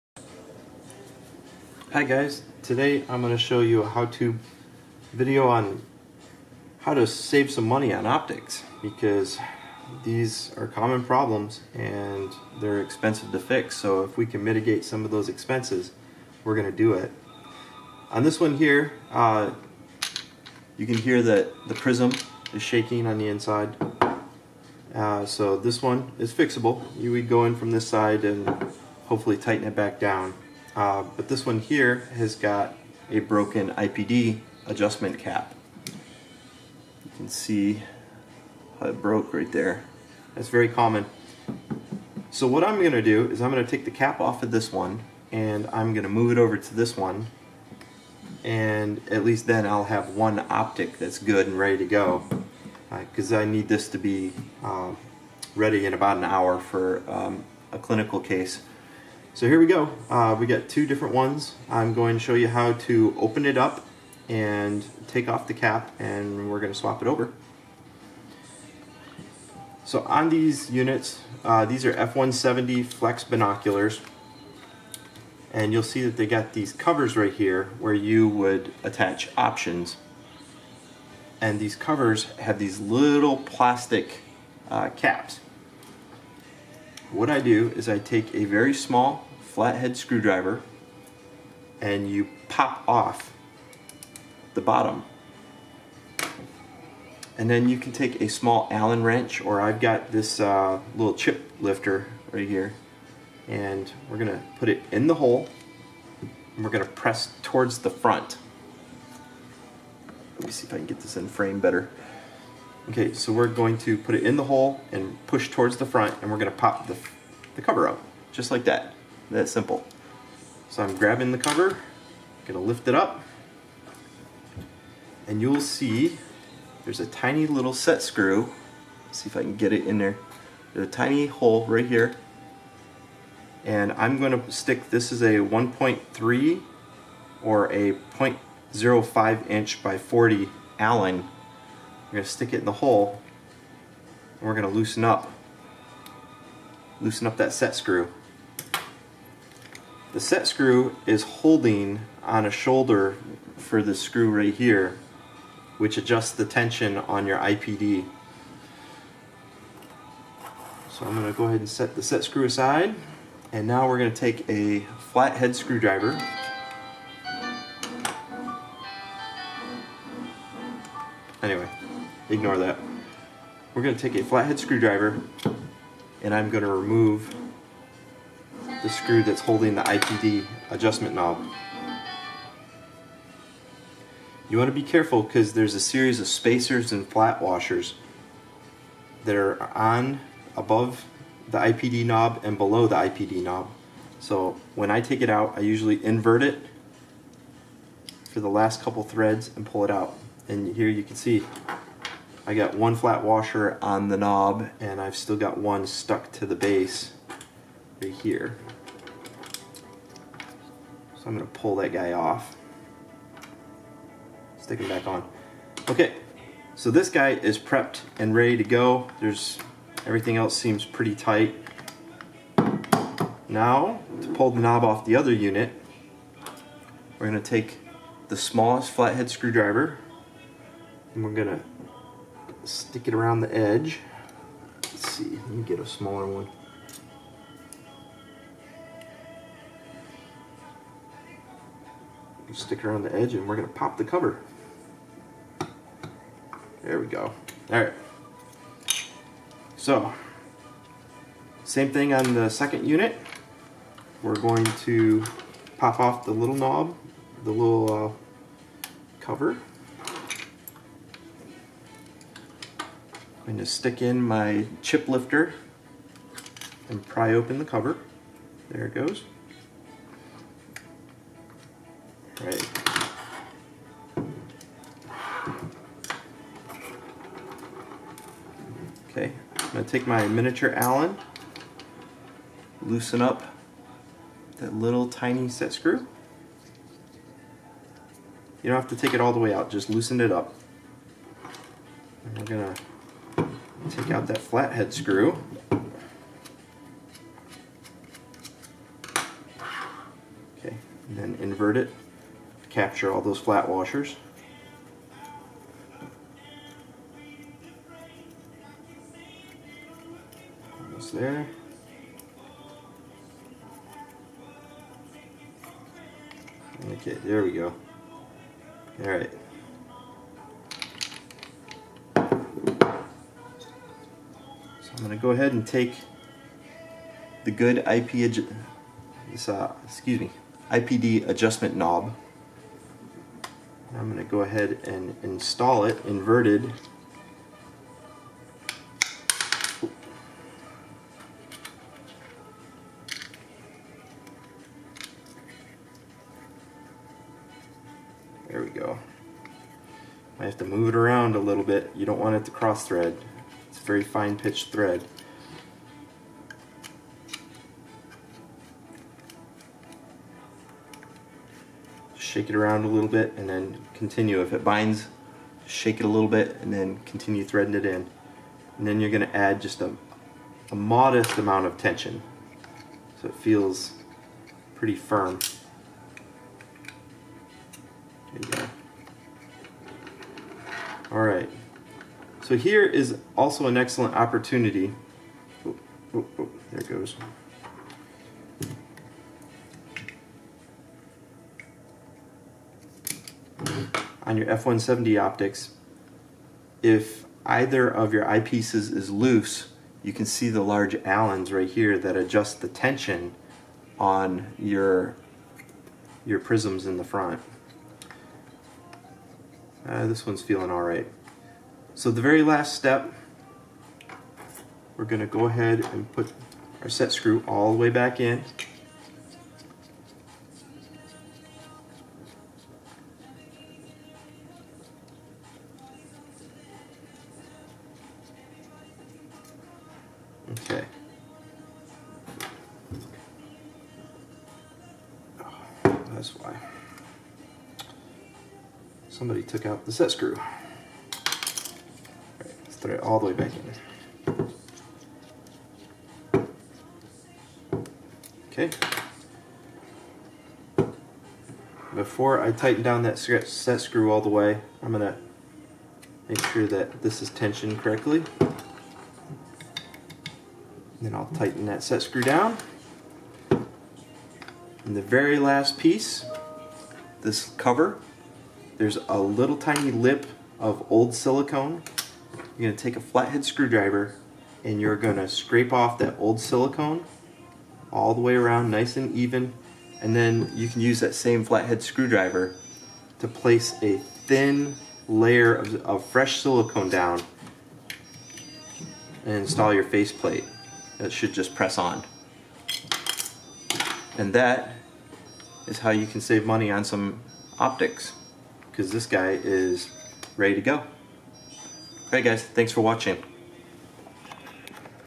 Zeiss F170 Flex Binocular Repair
This is a simple repair of a Zeiss F170 IPD Knob.
Please excuse the fact that I install the knob 180deg out of orientation. I hurried to make this video to help someone out.